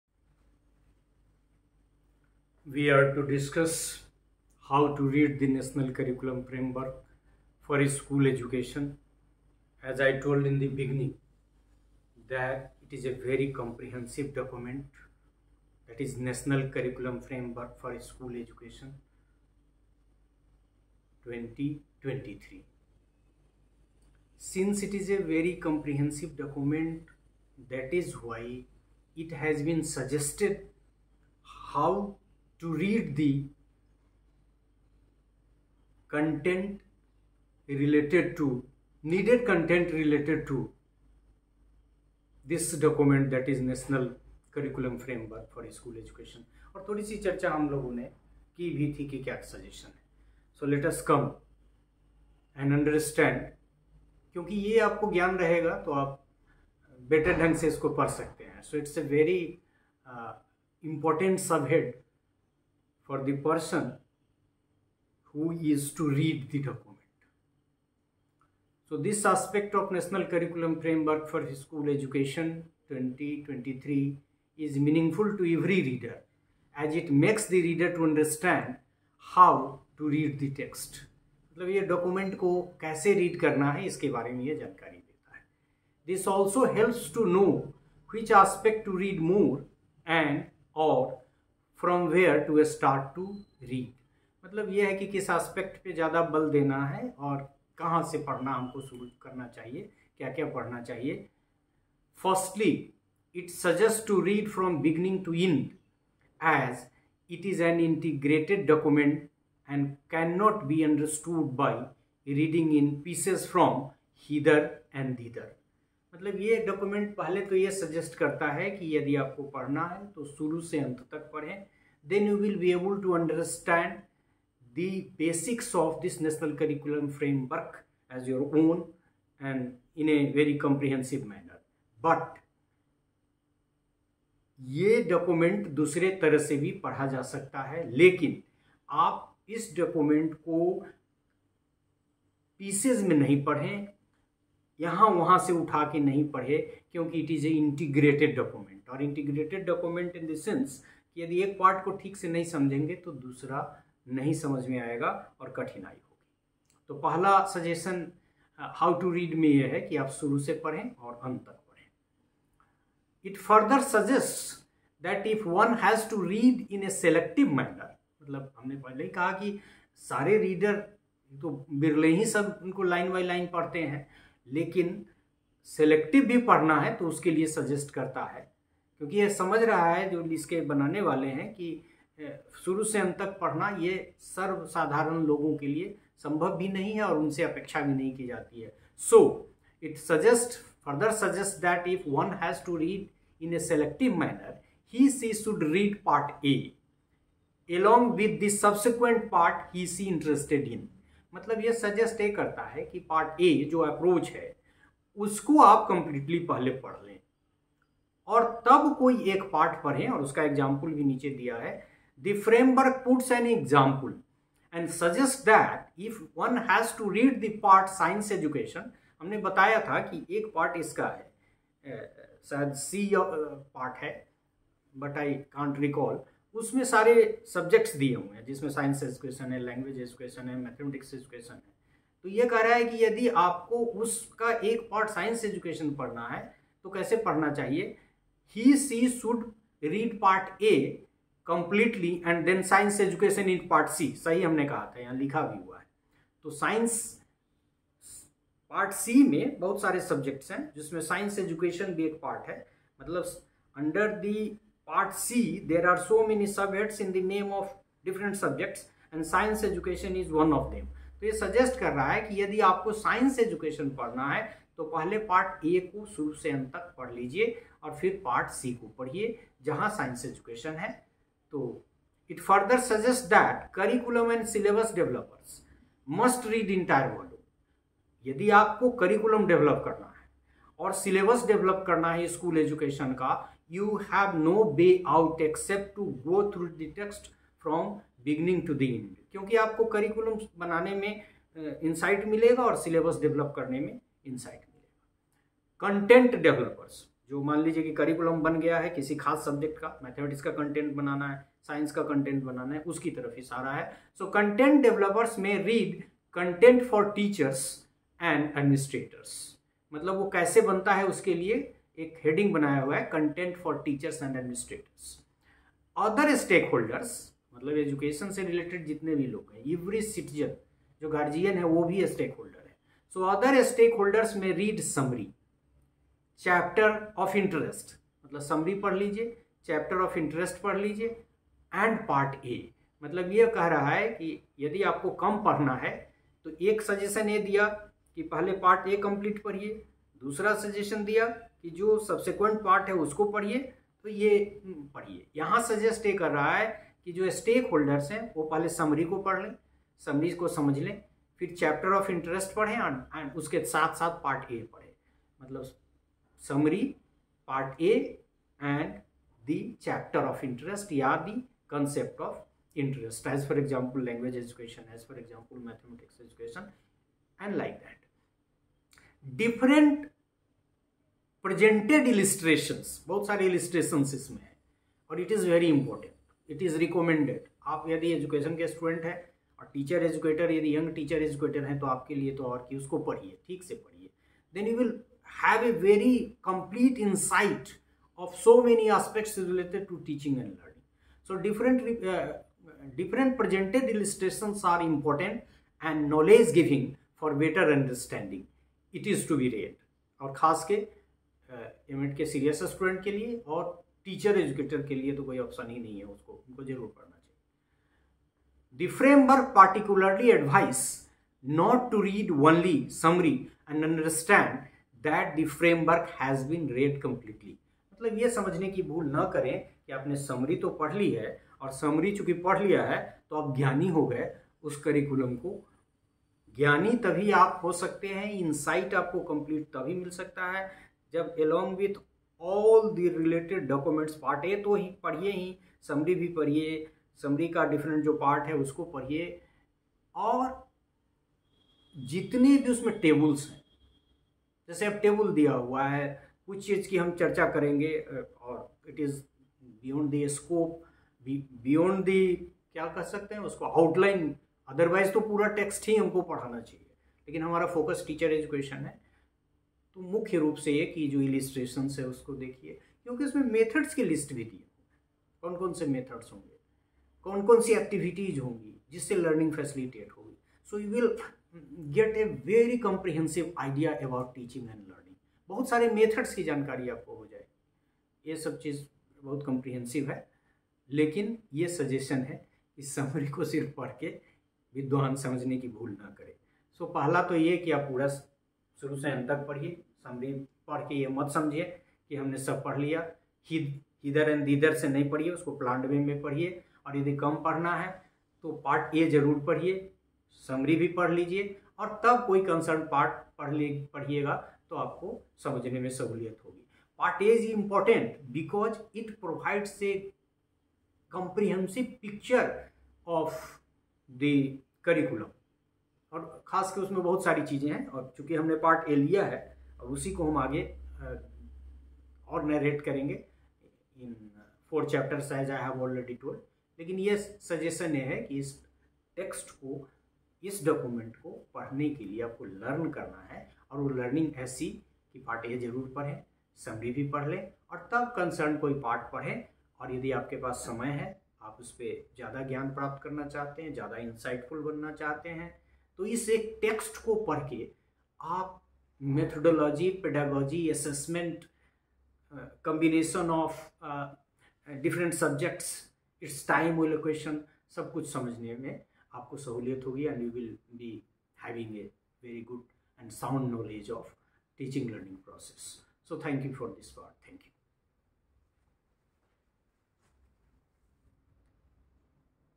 How to Read the National Curriculum Framework for School Education (Part - 3)
This lecture is delivered by Prof. Lalit Kumar, Faculty of Education, Patna University , and is dedicated to his wife Meera Kumari. The lecture is particularly important for all the individuals interested in educational issues. This lecture is specially useful for the Students of Education like B.Ed, M.Ed, M.A Education , and researchers.